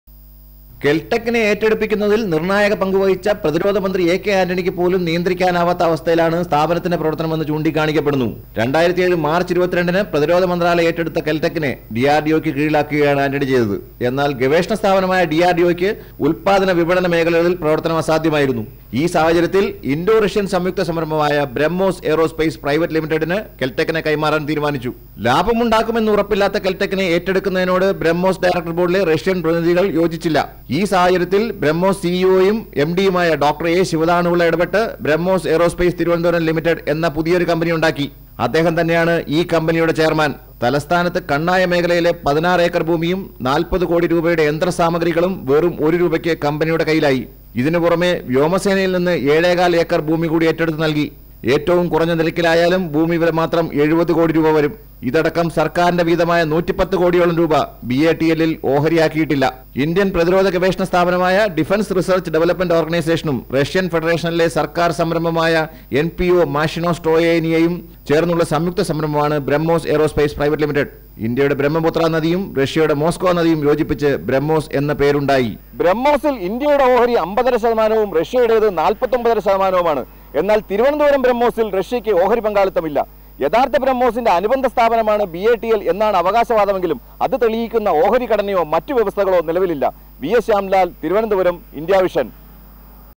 BrahMos Aerospace Thiruvananthapuram Ltd not part of original BrahMos
കെല്‍ടെക്കിനെ പ്രതിരോധ മന്ത്രാലയം ഏറ്റെടുത്തപ്പോള്‍ നിലവില്‍ വന്ന ബ്രഹ്മോസ് ഏറോസ്‌പേസ് തിരുവനന്തപുരം ലിമിറ്റഡ്, യഥാര്‍ത്ഥ ബ്രഹ്മോസിന്റെ ഭാഗമല്ലെന്നു വ്യക്തമായി. രണ്ടു വെവ്വേറെ കമ്പനികള്‍ രജിസ്റ്റര്‍ ചെയ്തിട്ടുള്ളതിന്റെ രേഖകള്‍ ഇന്ത്യാവിഷന് ലഭിച്ചു.